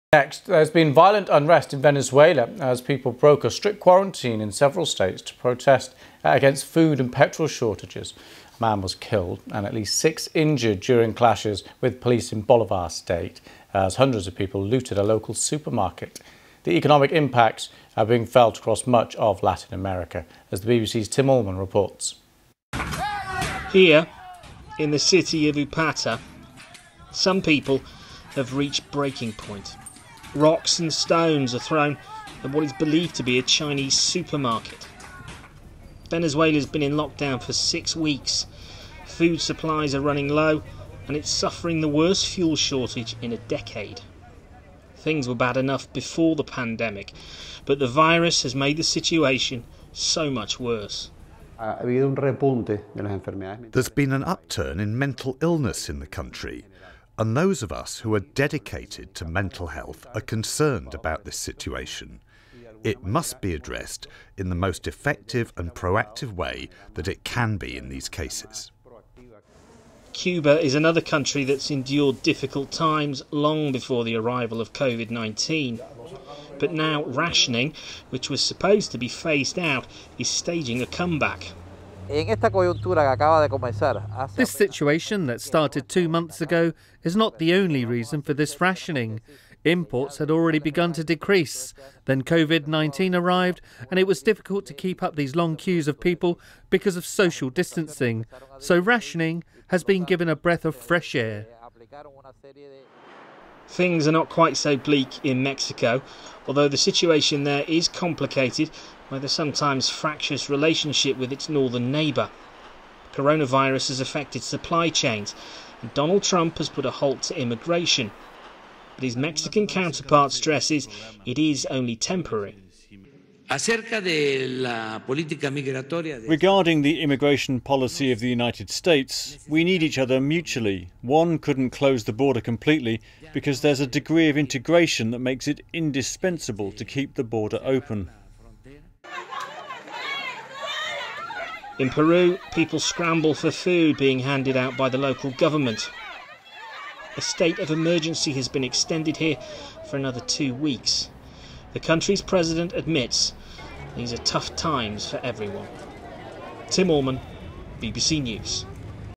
Looting takes place in Venezuela as the Coronavirus pandemic continues
There's been violent unrest in Venezuela as people broke a strict quarantine in several states to protest against food and petrol shortages. A man was killed and at least six injured during clashes with police in Bolivar state - as hundreds of people looted a local supermarket. The economic impact's being felt across much of Latin America - as the BBC's Tim Allman reports.